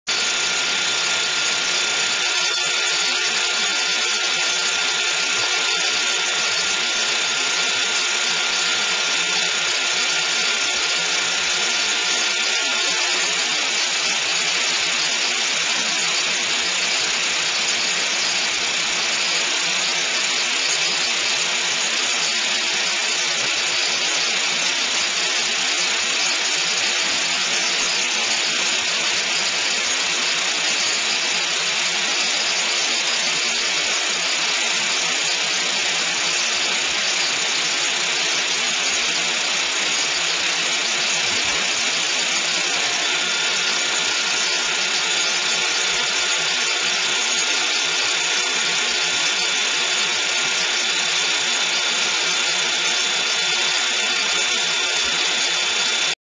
浮遊流輸送
浮遊流とは、輸送管内の風速が速く、粉粒体は飛翔しながら、移動するような流れであり、いわゆる低濃度輸送に属します。